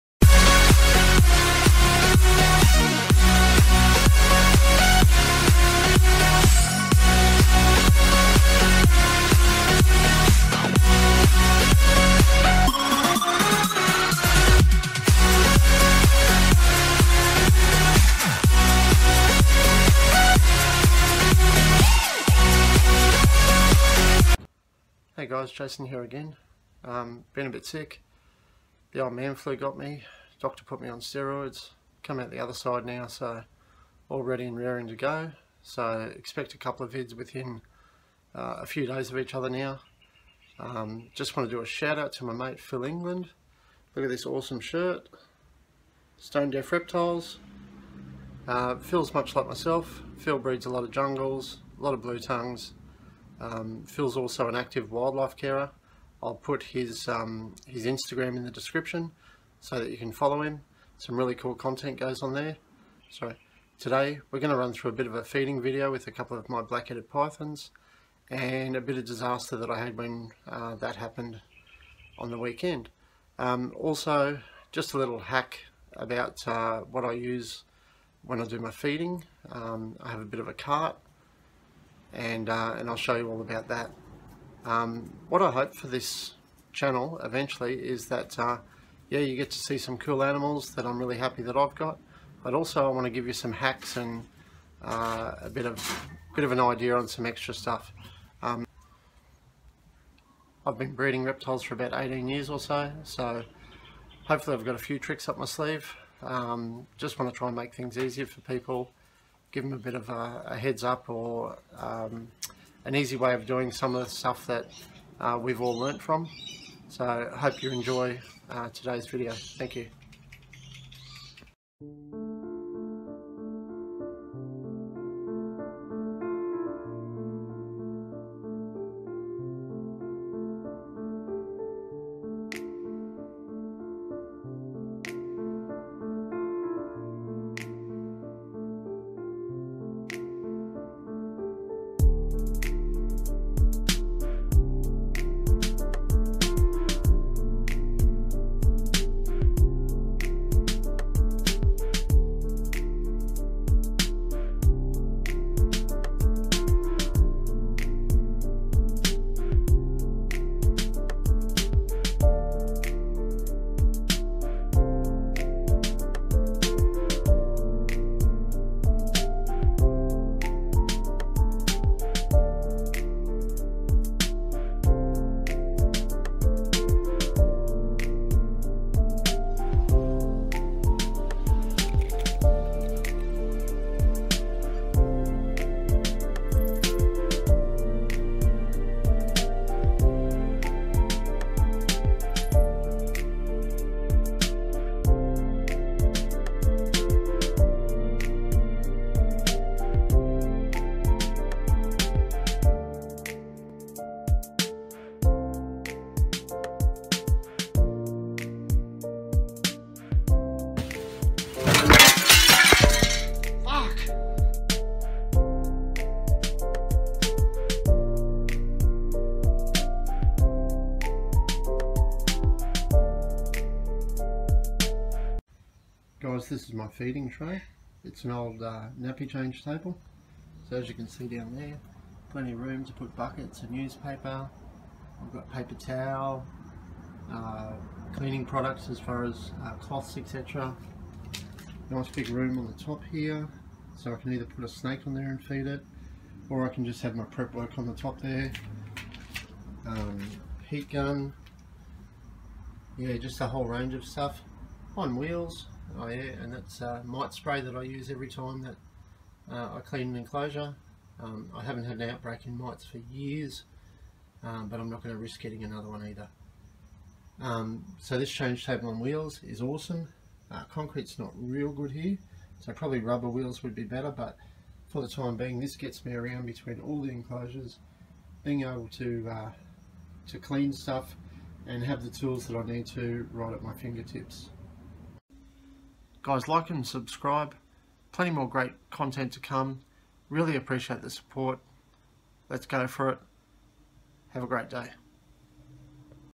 VLOG 4: Feeding Big Black Headed Pythons, and Disaster!
A Quick Feed vid with a BHP, DISASTER! and my feed cart.